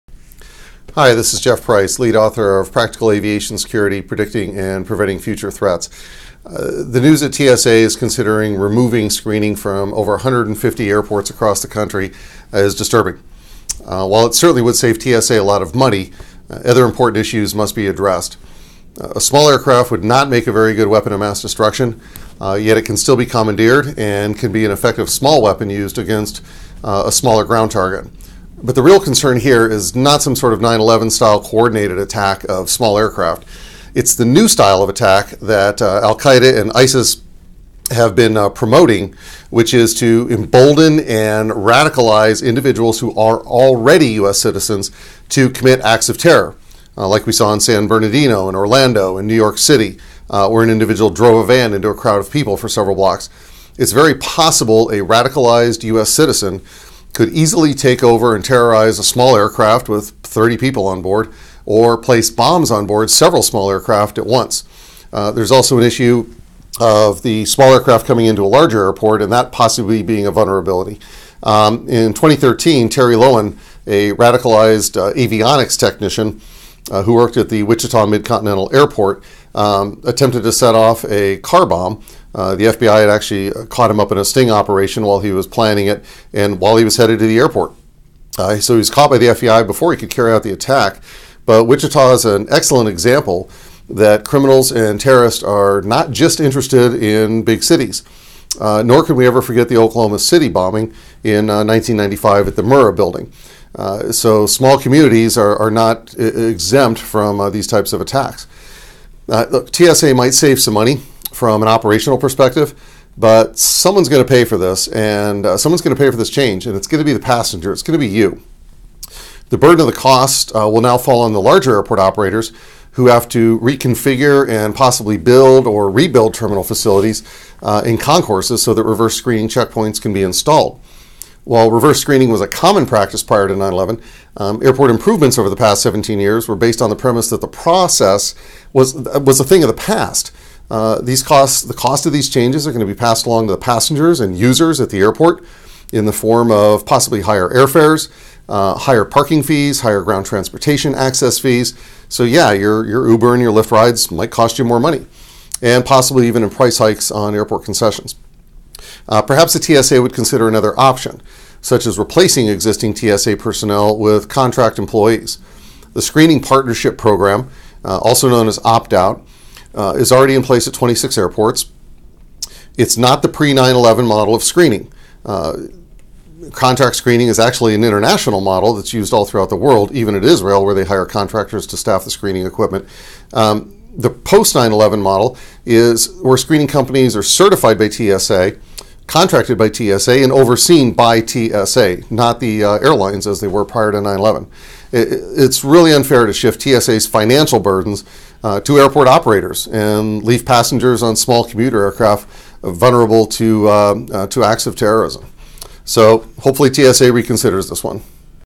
TSA to eliminate screening from small airports
Response to TSA proposal to eliminate passenger and baggage screening on smaller airplanes.